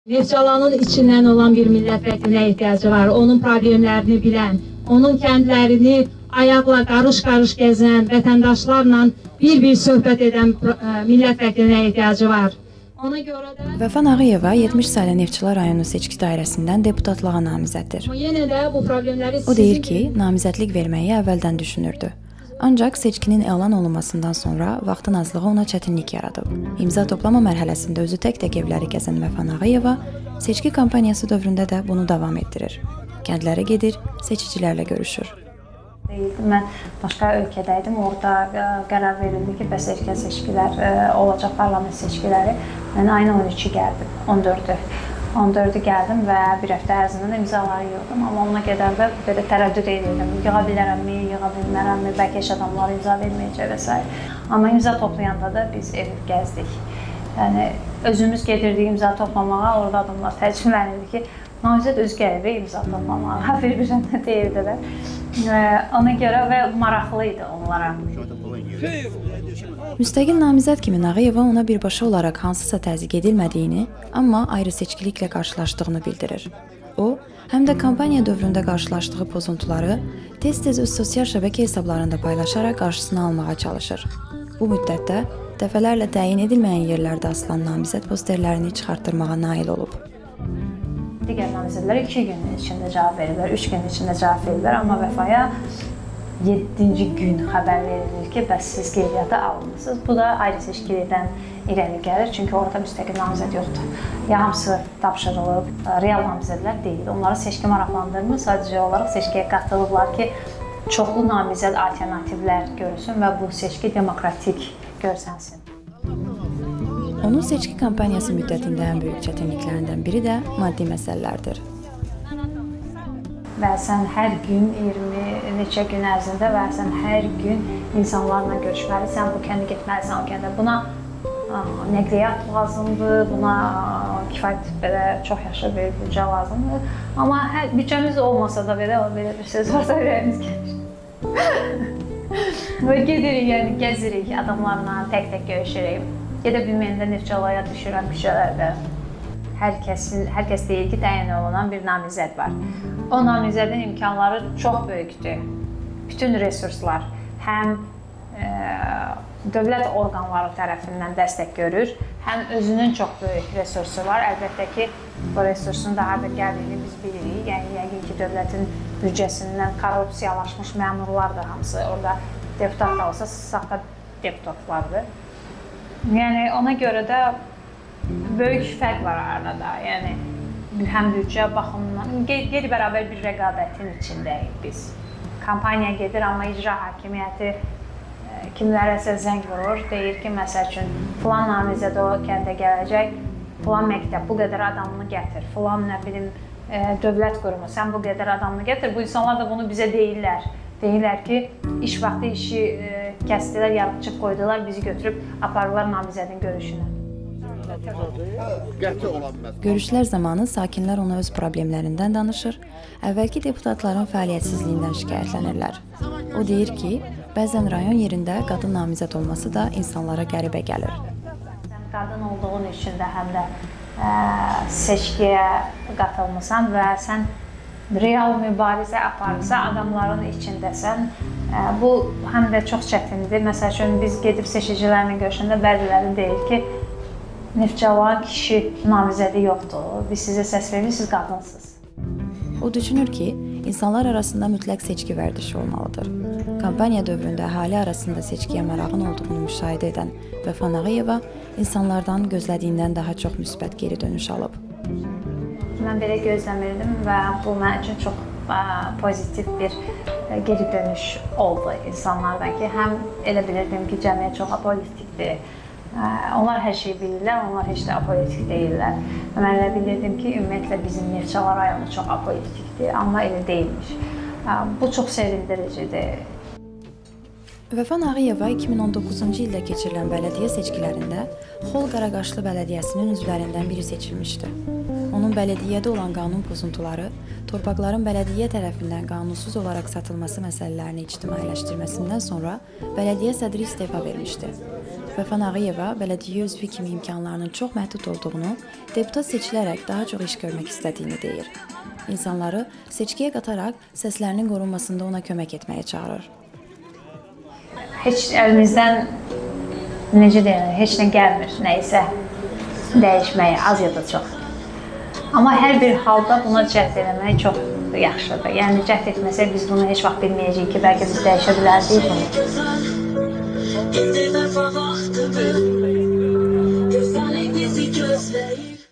Müstəqil namizəd olmağın çətinlikləri
Vəfa Nağıyeva 70 saylı Neftçala rayonu seçki dairəsindən deputatlığa namizəddir.

İmza toplama mərhələsində tək-tək evləri gəzən Vəfa Nağıyeva seçki kompaniyası dövründə də bunu davam etdirir.